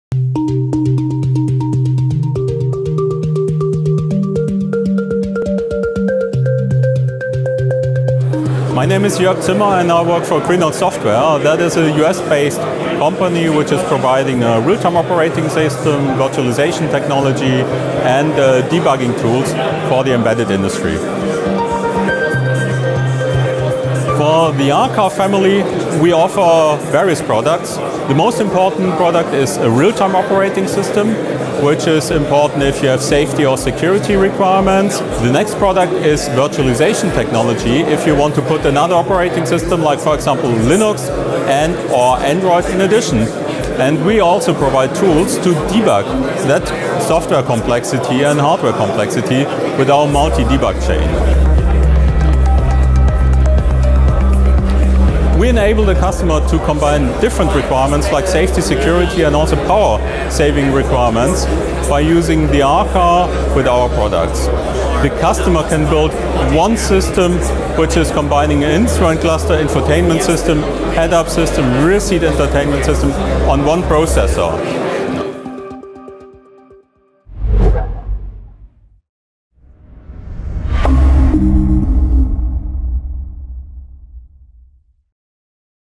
Green Hills Software R-Car Solutions
Green Hills INTEGRITY Multivisor CANVAS demo on R-Car H3:
• INTEGRITY RTOS with INTEGRITY Multivisor hosting one Linux guest on a single R-Car H3
• Rightware Kanzi and rear-view camera on INTEGRITY
• Yocto GENIVI Linux with 3 touchscreens running Wayland
• HW-assisted GPU sharing between Linux and INTEGRITY
• Fast boot, Safe and Secure consolidation platform